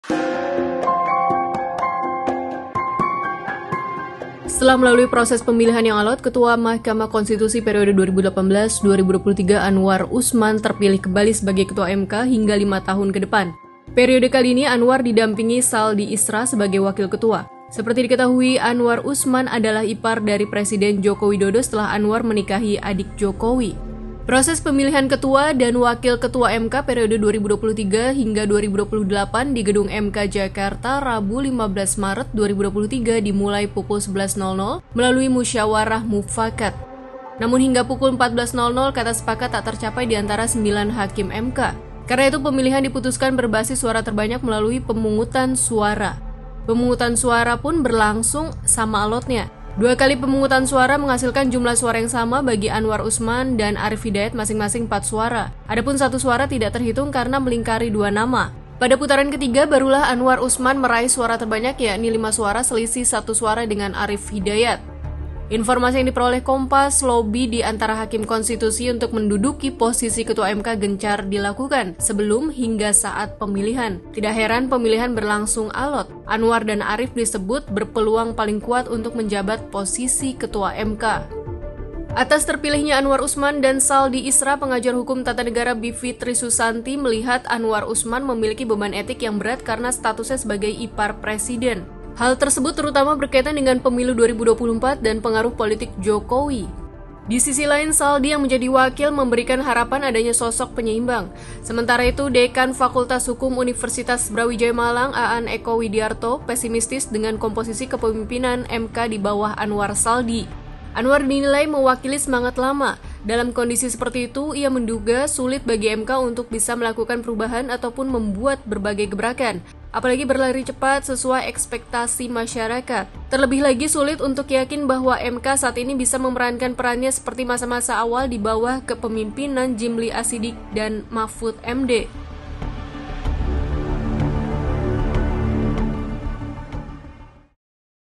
Ipar Jokowi Anwar Usman Dua Periode Ketua, MK Harus Pulihkan Kepercayaan Publik
Setelah melalui proses pemilihan yang alot, Ketua Mahkamah Konstitusi periode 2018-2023 Anwar Usman terpilih kembali sebagai Ketua MK hingga lima tahun ke depan. Periode kali ini, Anwar didampingi Saldi Isra sebagai wakil ketua. Seperti diketahui, Anwar Usman adalah ipar dari Presiden Joko Widodo setelah Anwar menikahi adik Jokowi.

Proses pemilihan ketua dan wakil ketua MK periode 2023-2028, di Gedung MK, Jakarta, Rabu (15/3/2023), dimulai pukul 11.00 melalui musyawarh mufakat. Namun, hingga pukul 14.00, kata sepakat tak tercapai di antara sembilan hakim MK. Karena itu, pemilihan diputuskan berbasis suara terbanyak melalui pemungutan suara. Pemungutan suara pun berlangsung sama alotnya. Dua kali pemungutan suara menghasilkan jumlah suara yang sama bagi Anwar Usman dan Arief Hidayat masing-masing 4 suara. Adapun satu suara tidak dihitung karena melingkari dua nama.
Pada putaran ketiga, barulah Anwar Usman meraih suara terbanyak yakni 5 suara, selisih satu suara dengan Arief Hidayat.

Informasi yang diperoleh Kompas, lobi di antara hakim konstitusi untuk menduduki posisi ketua MK gencar dilakukan sebelum hingga saat pemilihan. Tidak heran, pemilihan berlangsung alot. Anwar dan Arief disebut berpeluang paling kuat untuk menjabat posisi ketua MK.

Atas terpilihnya Anwar Usman dan Saldi Isra, pengajar hukum tata negara Bivitri Susanti melihat Anwar Usman memiliki beban etik yang berat karena statusnya sebagai ipar Presiden. Hal tersebut terutama berkaitan dengan Pemilu 2024 dan pengaruh politik Jokowi. Di sisi lain, Saldi yang menjadi wakil memberikan harapan adanya sosok penyeimbang.
Sementara itu, Dekan Fakultas Hukum Universitas Brawijaya, Malang, Aan Eko Widiarto, pesimistis dengan komposisi kepemimpinan MK di bawah Anwar-Saldi. Anwar dinilainya mewakili semangat lama. Dalam kondisi seperti itu, ia menduga sulit bagi MK untuk bisa melakukan perubahan ataupun membuat berbagai gebrakan, apalagi berlari cepat sesuai ekspektasi masyarakat. Terlebih lagi, sulit untuk yakin bahwa MK saat ini bisa memerankan perannya seperti masa-masa awal di bawah kepemimpinan Jimly Asshiddiqie dan Mahfud MD.

Dapatkan akses gratis selama 3 bulan, tanpa biaya langganan. Klaim aksesnya sekarang juga